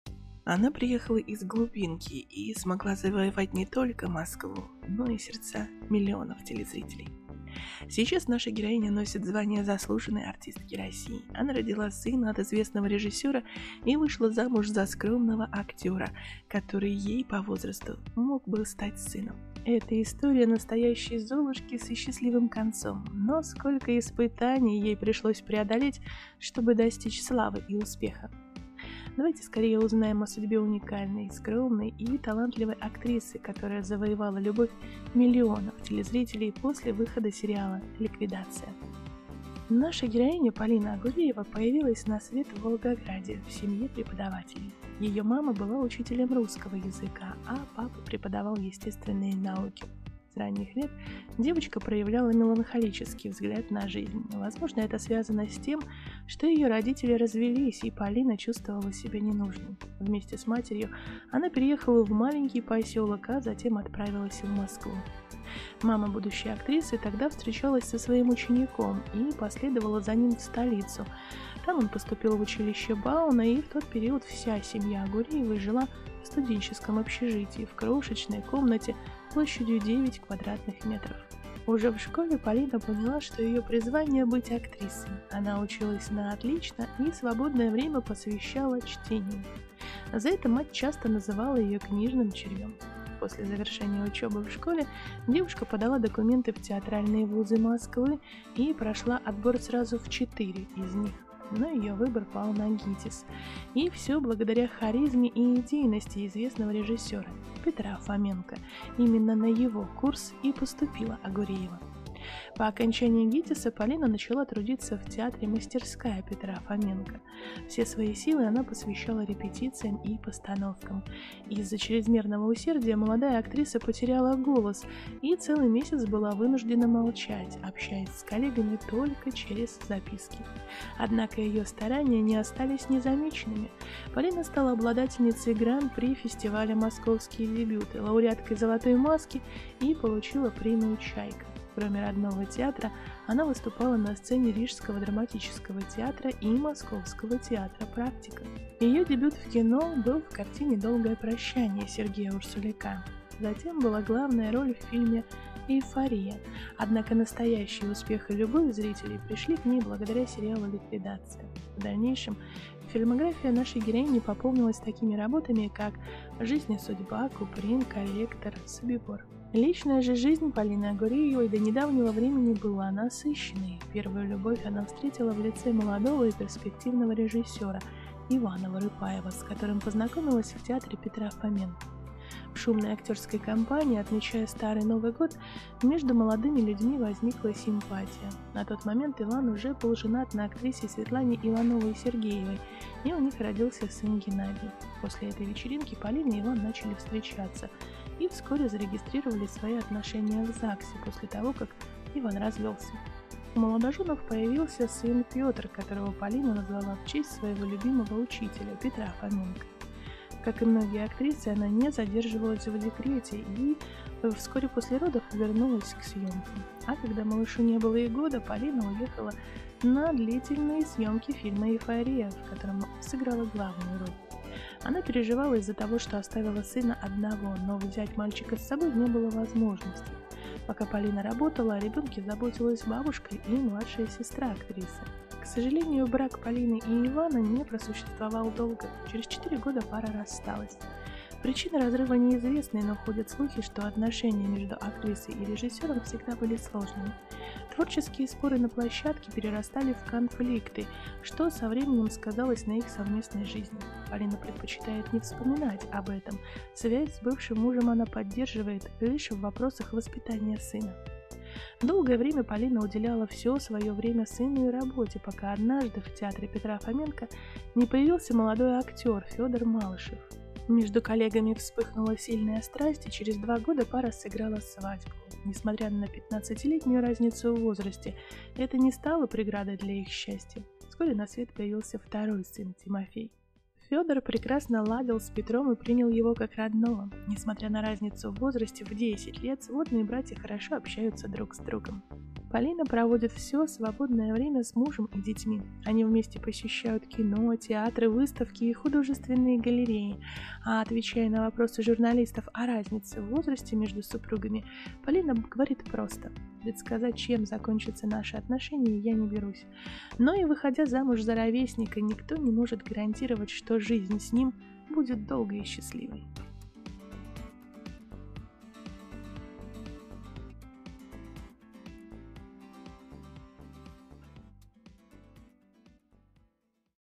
Ей 48, а мужу 33: Родила от знаменитого кинорежиссера, а потом от скромного актёра. Полина Агуреева
Ей 48, а мужу 33: Родила от знаменитого кинорежиссера, а потом от скромного актёра. Полина Агуреева личная жизнь биография